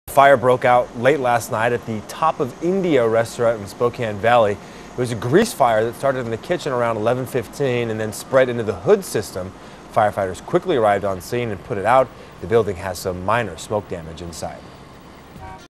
Fire breaks out at Spokane Valley restaurant
A fire broke out at the "Top of India" restaurant in the Spokane Valley late Tuesday night.